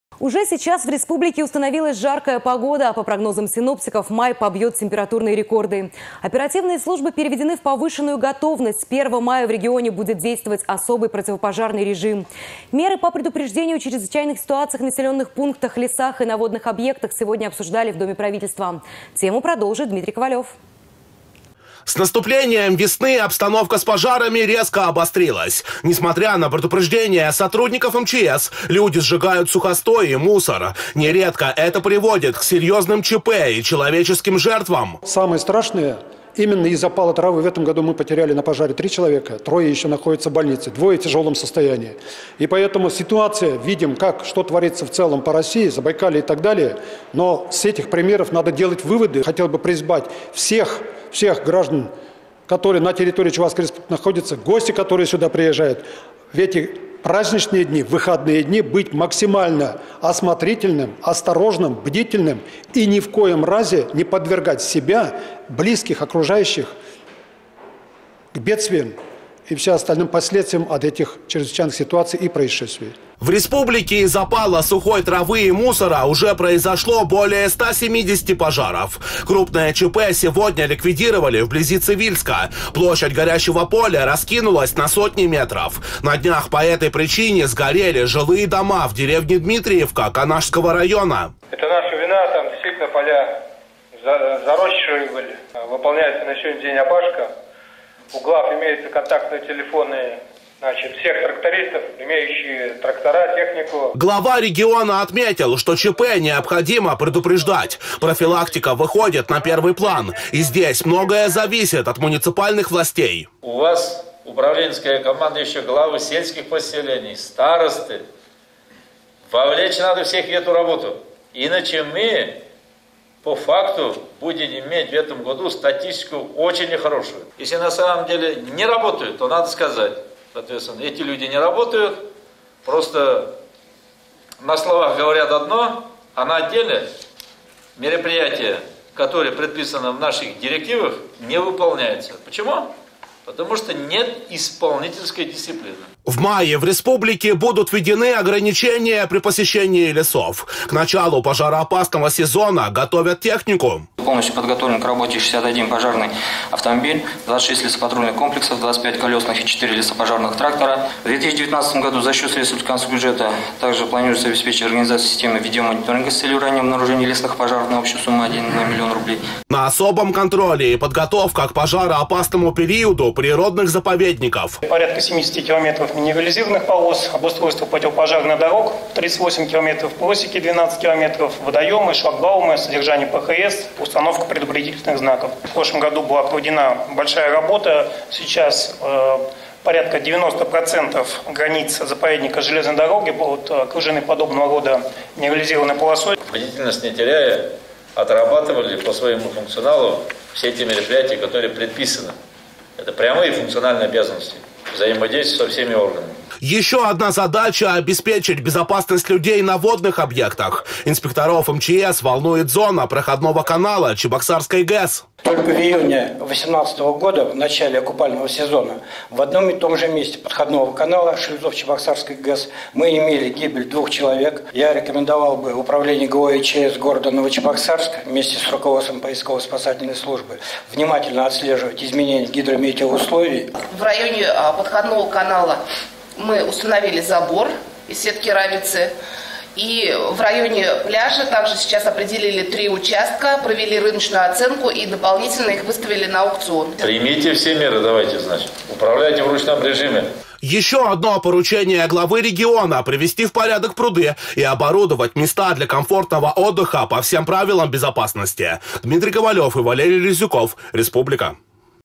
Уже сейчас в республике установилась жаркая погода.
Оперативные службы переведены в повышенную готовность. С 1 мая в регионе будет действовать особый противопожарный режим. Меры по предупреждению чрезвычайных ситуаций в населенных пунктах, лесах и на водных объектах сегодня обсуждали в Доме правительства.